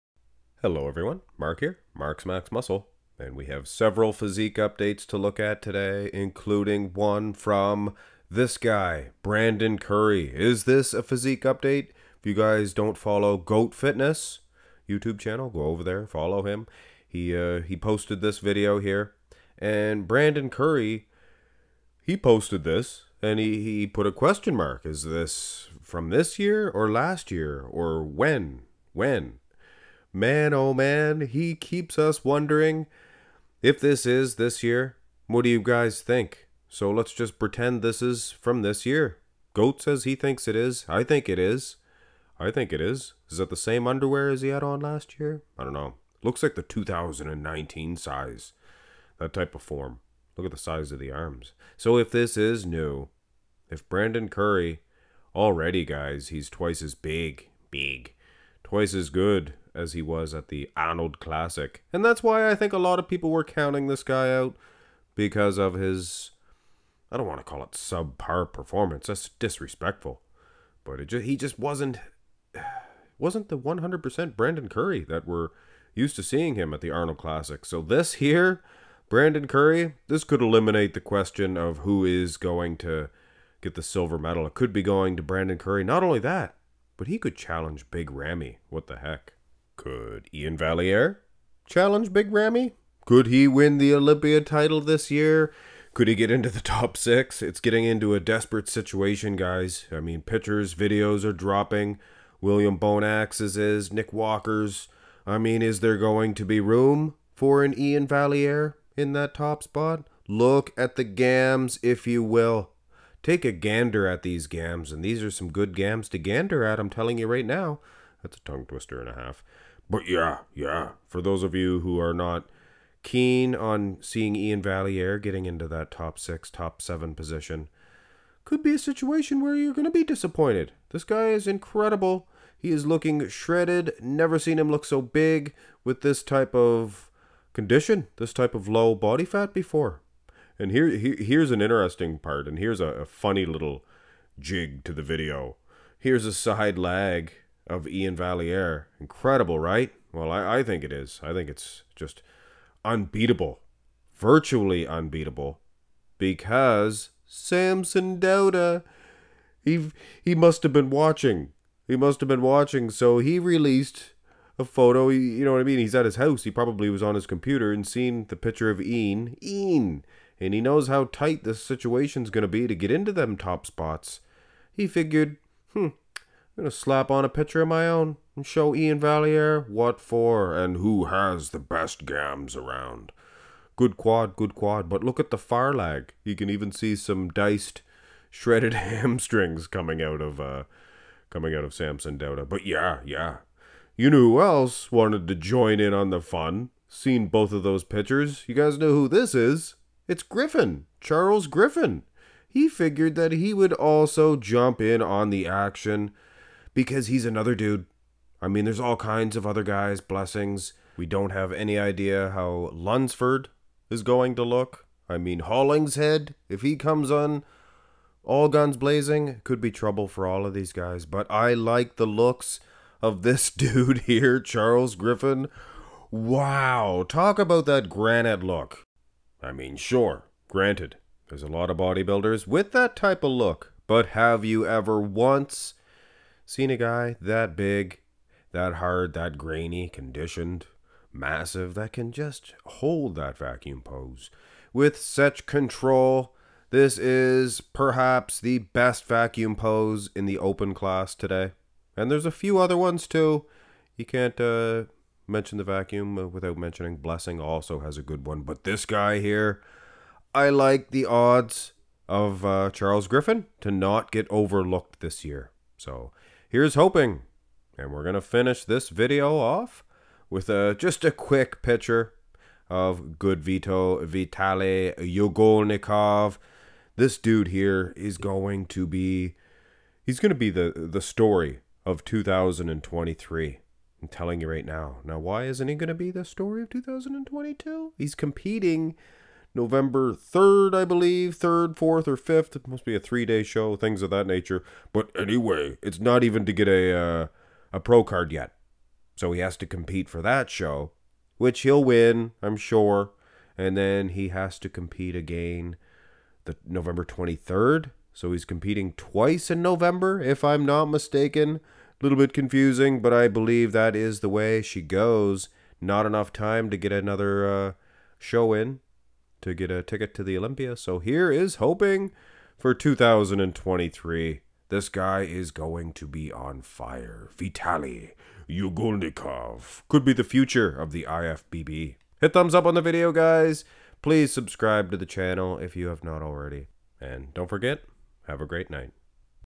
Update#34*Brandon Curry*Iain Valliere*Samson Dauda*Charles Griffen*Goodvito*
Todays update is full of big names such as the former Mr. Olympia Brandon Curry, and many more.
Timestamps
0:00 Brandon Curry
1:30 Iain Valliere
2:36 Samson Dauda
3:15 Charles Griffen
4:30 Goodvito